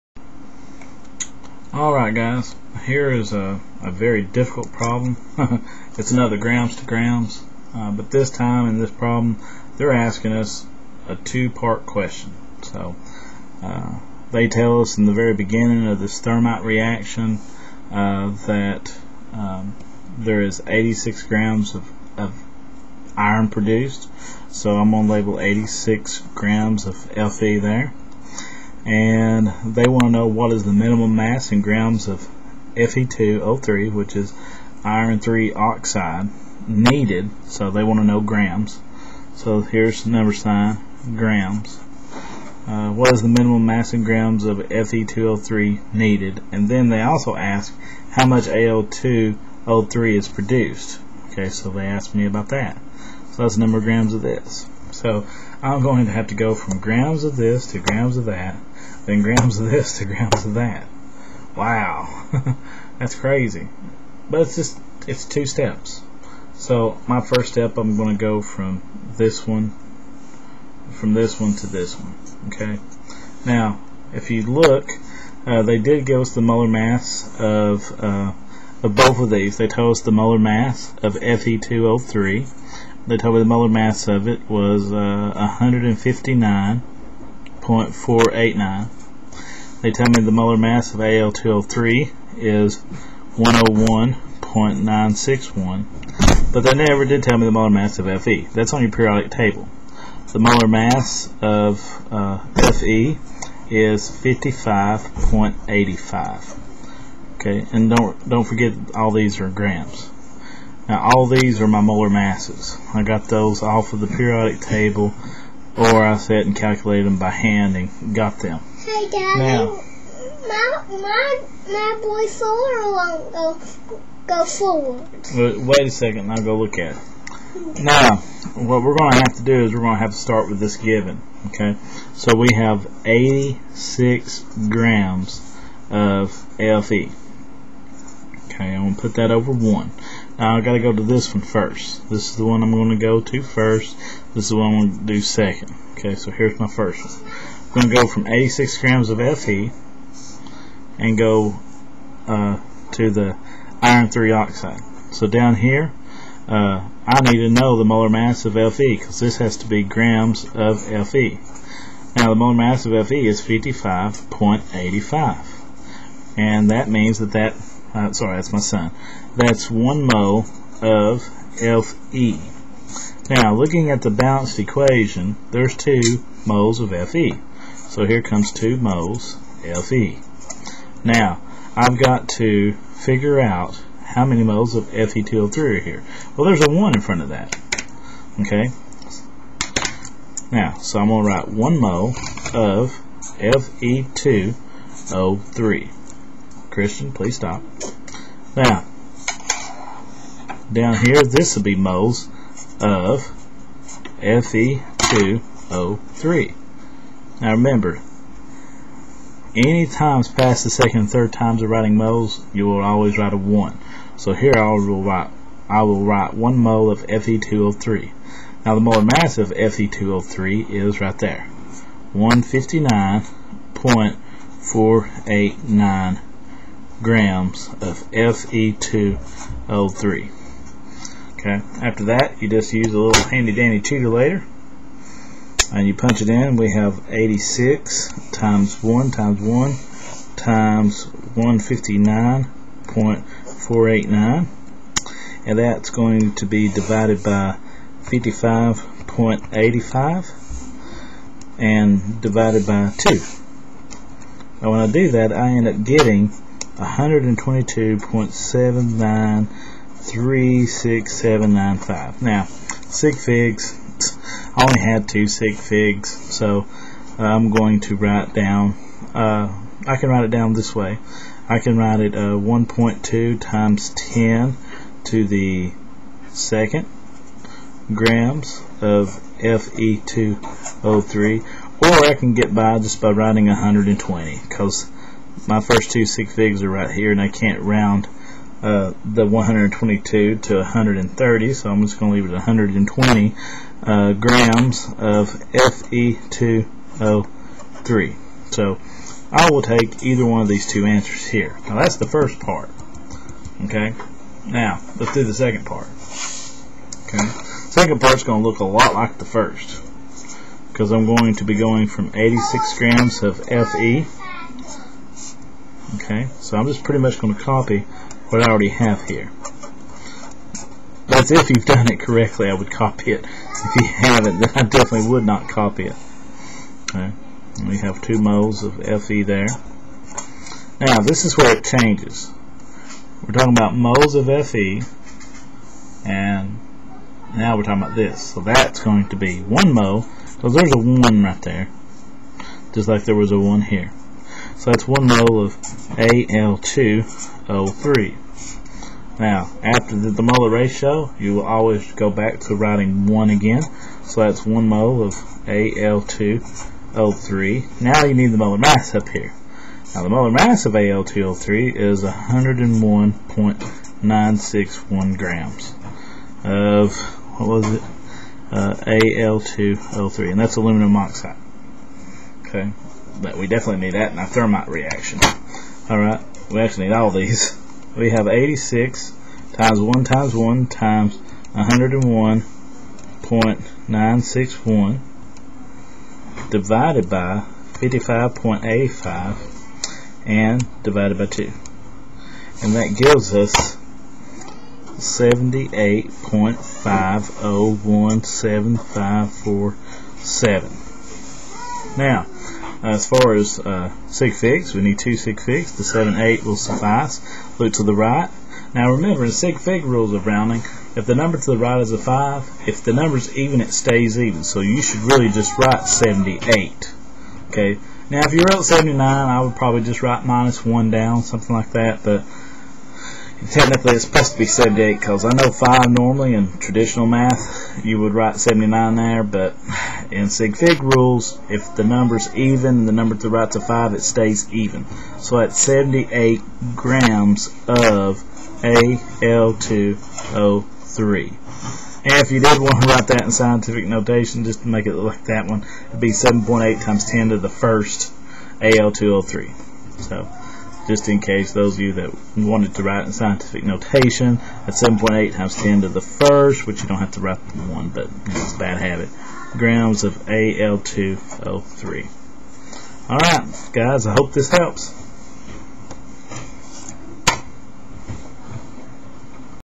Stoichiometry - Grams to Grams (using a balanced equation) Video #2
Stoichiometry - Grams to Grams (using a balanced equation) Video #2| Chemistry Whitwell High School UTC - University of Tennessee at Chattanooga Instructor/Professor: Johnny Cantrell